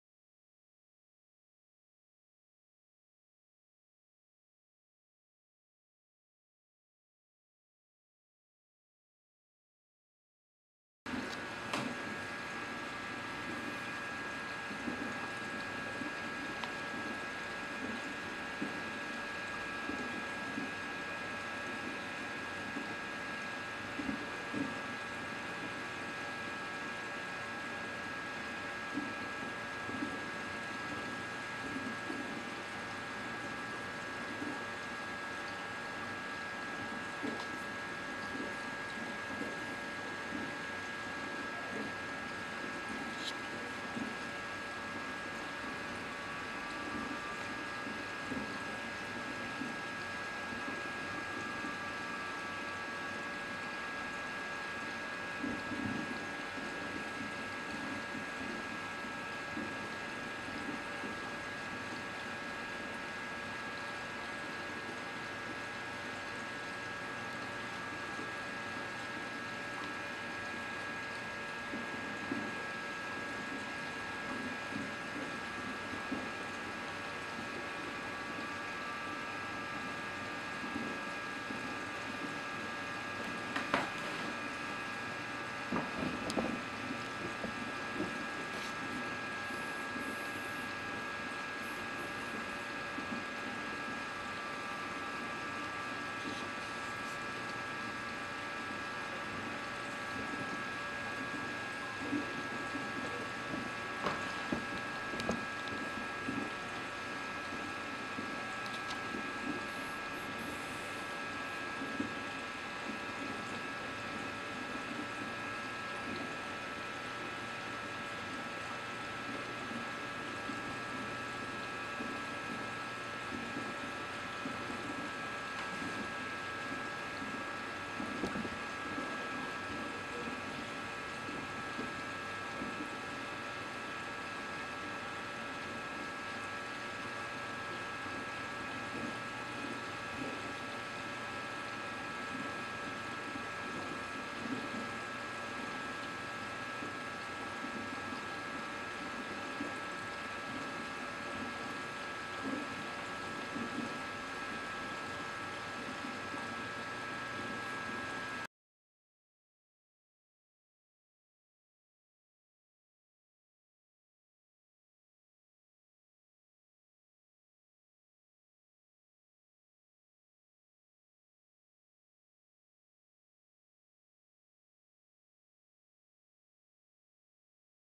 Mexican Turbo Snail
Mexican Turbo Snail eating green algae from the aquarium glass